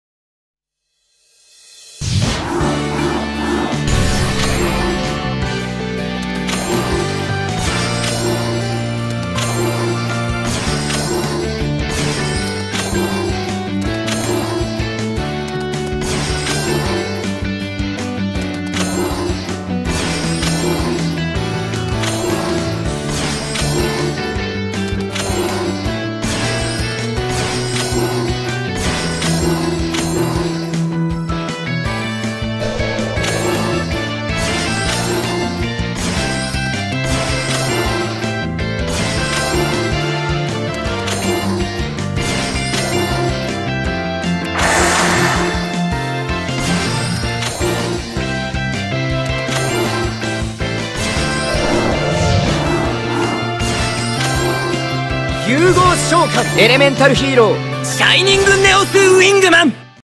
BEST 2 CARDS COMBO NEW FLAME WINGMAN!
This combo is really strong Dark Angel + Law + DPE and SNW so much interrupton New Wingman is really good...